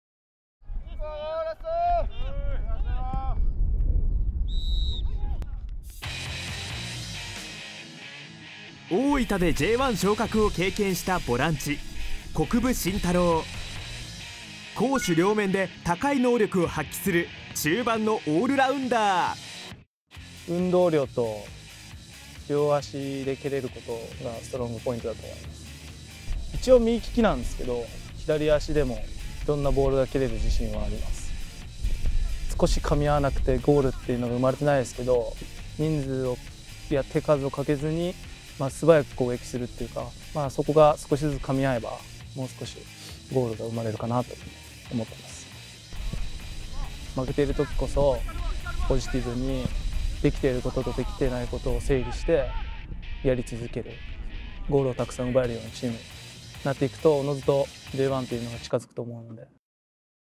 MONTEDIO PLAYERS ④國分伸太郎
モンテディオ山形　國分伸太郎 選手　今季に懸ける思い
MONTEDIO　PLAYERS